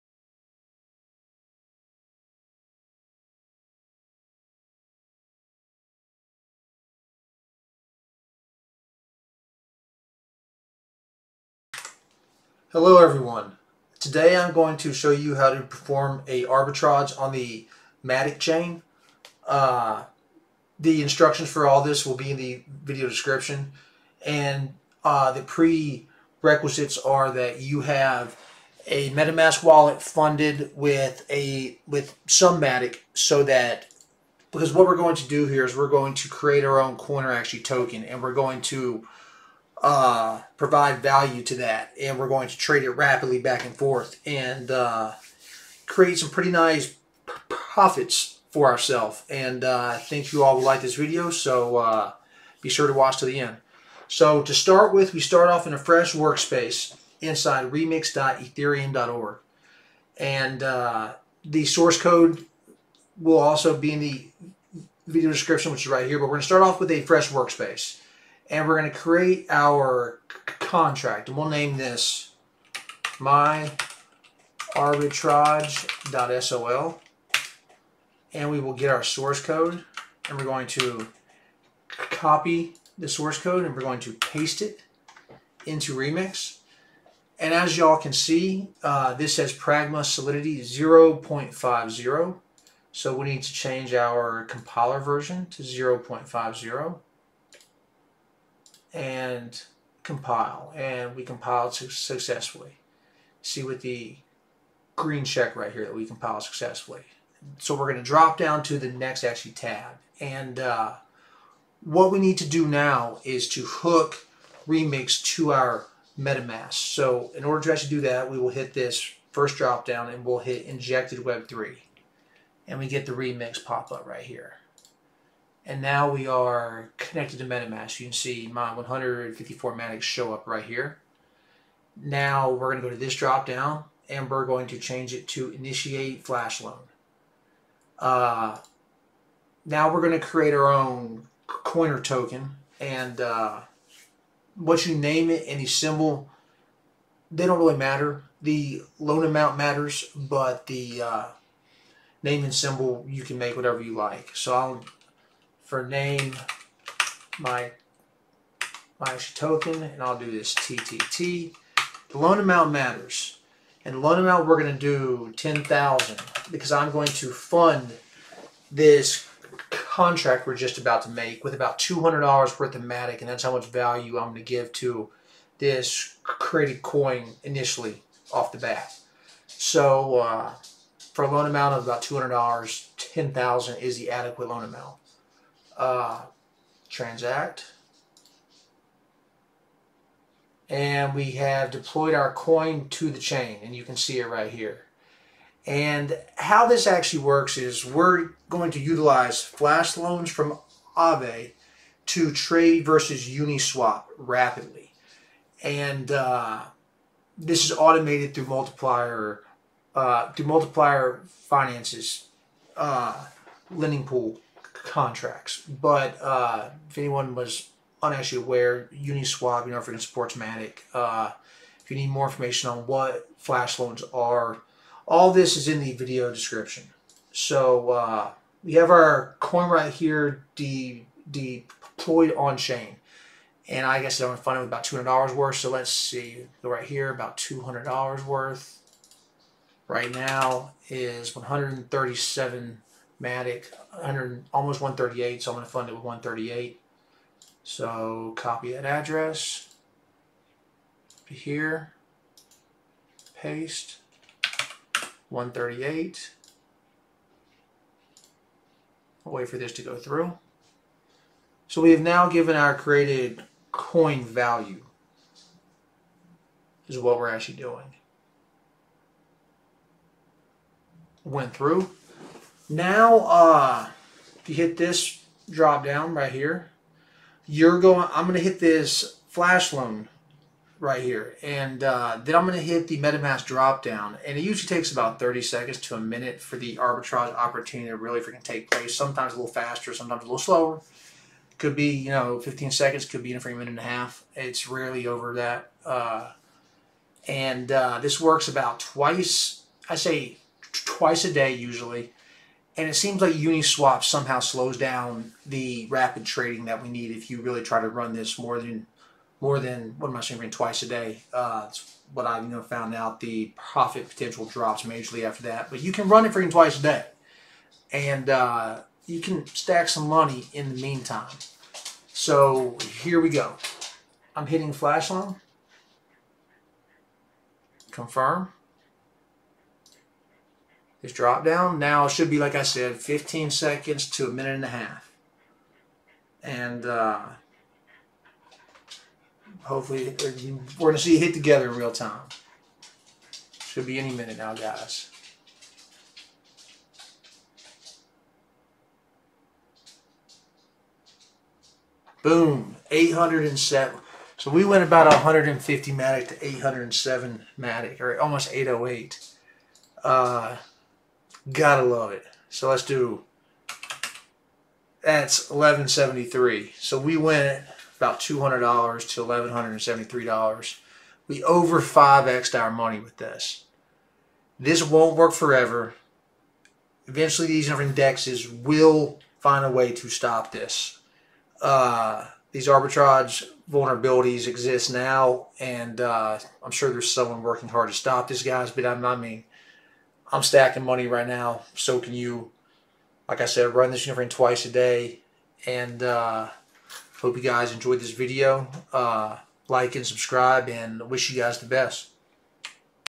Matic - Polygon Chain: Insane Multi DEX arbitrage attack on Polygon with Metamask and Solidity
NOTE: "Web 3 Injected" is now "Injected Provider" in Remix.

TO EARN MORE THAN THE AMOUNT IN THE VIDEO YOU CAN INCREASE GAS FEE

In this video, you can see how I used Flashloans to Arbitrage in Matic Mainnet by deploying Contract/Token to mask our arbitrage attack against AAVE.

This worked for me. But TRY IT AT YOUR OWN RISK.

STEPS:

Before starting, install and setup MetaMask in Chrome Browser.

1. Open and setup Remix in browser

2. Copy the Contract code

3. Create new file in Remix and paste Contract code and wait for everything to load, Go to Solidity Compiler and Select compiler to 0.5.0+commit version and hit Compile.

4. In Deploy & Run Transactions, Select Injected Web3 in Environment dropdown menu to connect your MetaMask. On Deploy, Enter your token details and deploy the contract

5. Get the Contract address from Remix

6. Send 200$ish of MATIC to the contract for gas fee for all swaps (There are going to be many swaps to use all funds from out liquidity pools.

7. Call 'flashloan' function in Remix.